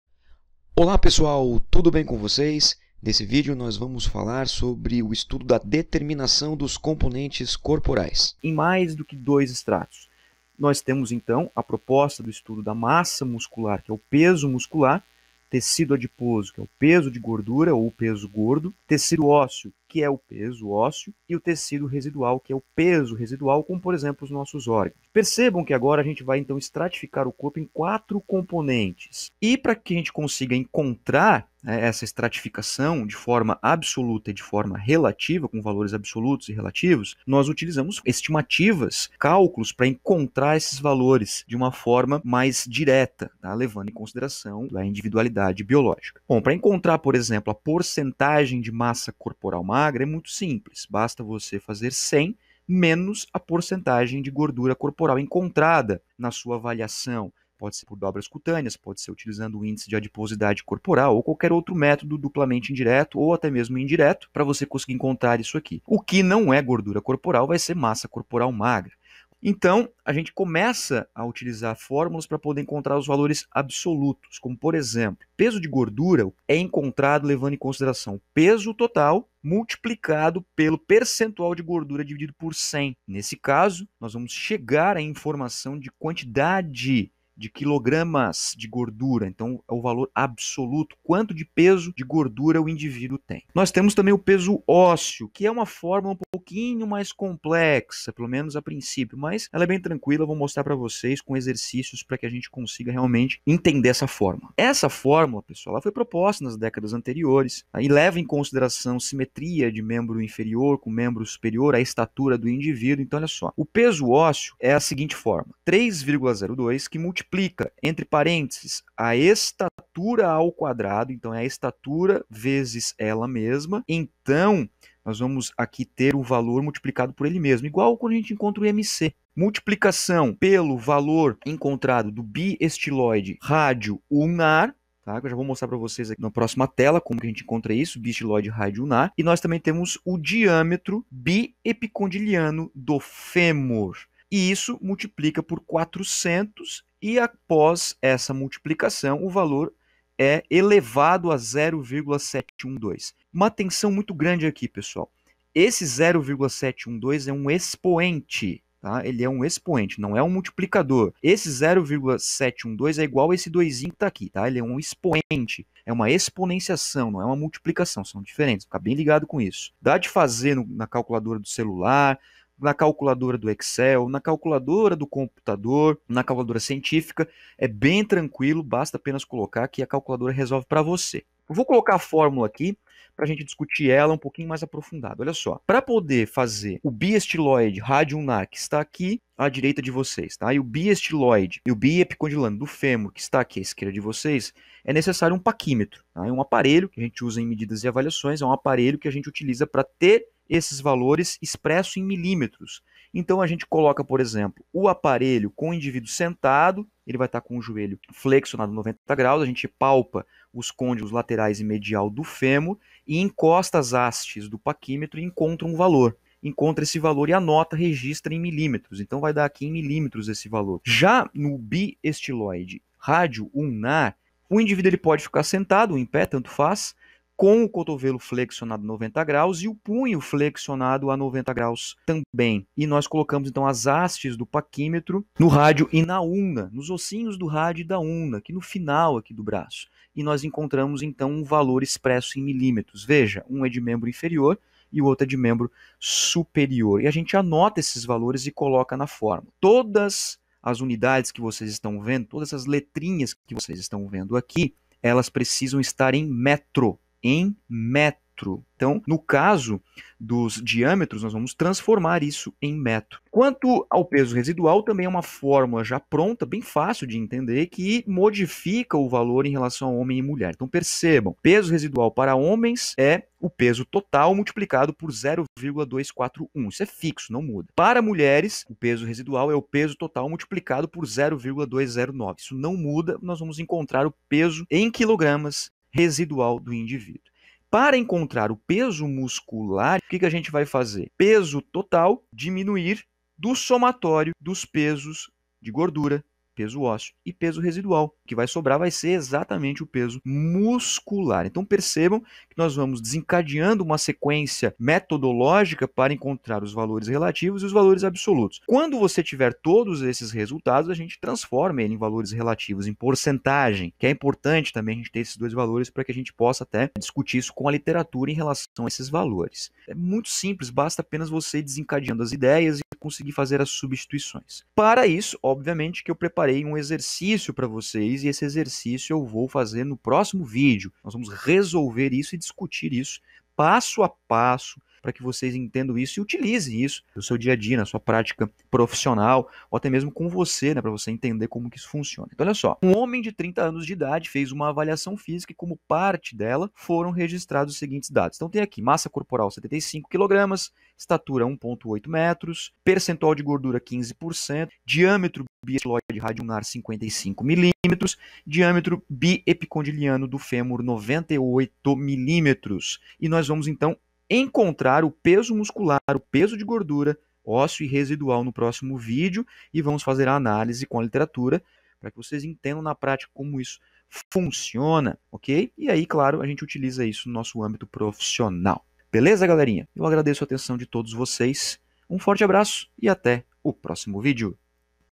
Estratificação da composição corporal em 4 componentes
A avaliação da composição corporal é realizada para quantificar os principais componentes do organismo humano: peso muscular, peso ósseo, peso de gordura e peso residual. O estudo da composição corporal é importante tanto para o estado geral de saúde quanto para o desempenho e performance.

Dessa forma, a análise da composição corporal possibilita compreender as modificações resultantes de alterações metabólicas e identificar riscos à saúde e auxiliar na prescrição do exercício físico.

É importante conhecer estratégias e ferramentas para o estudo de diversos componentes do corpo humano e com o profissional da saúde pode trabalhar com tais informações retiradas de uma avaliação, a fim de identificar quais as reais necessidades das pessoas, pois nem sempre seus objetivos ou anseios coincidem com o que realmente precisam.